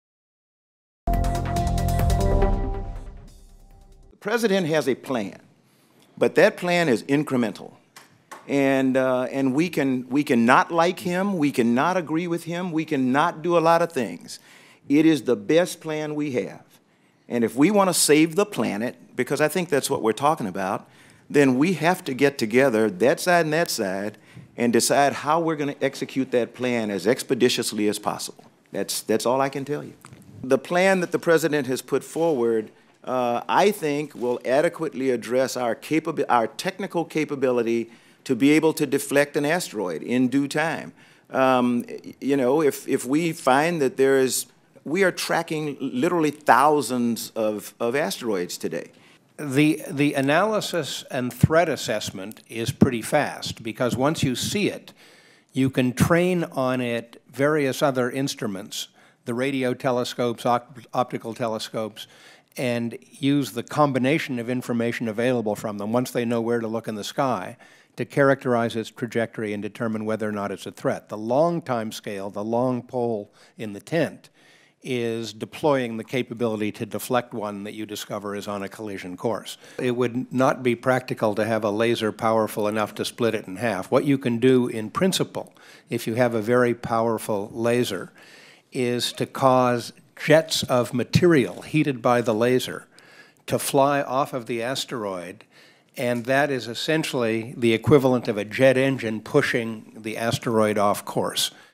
NASA & White House Discuss Asteroid Threat
NASA Administrator Charles Bolden and White House science chief John Holdren appeared before a House committee Tuesday to discuss how best to deflect incoming asteroids. Photo: AP.